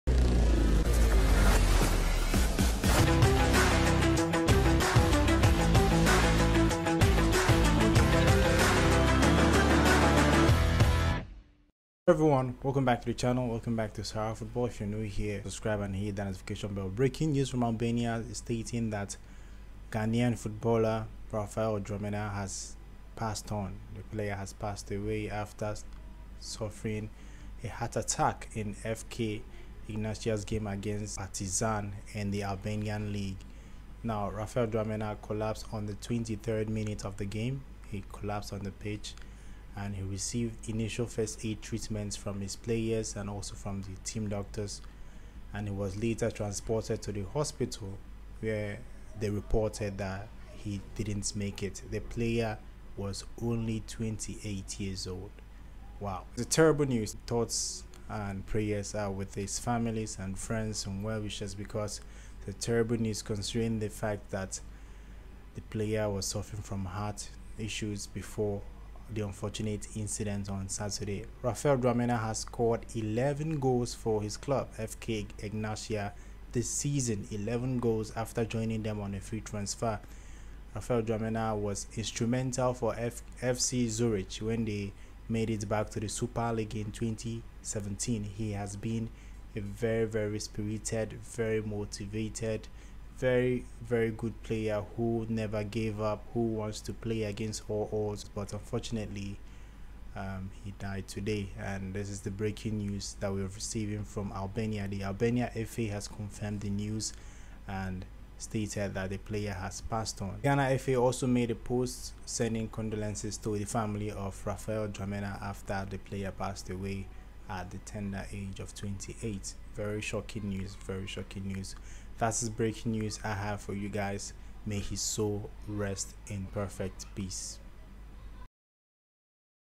GHANAIAN FOOTBALLER RAPHAEL DWAMENA DIES DURING FOOTBALL MATCH IN ALBANIA
Ghana footballer Raphael Dwamena dies at the age of 28.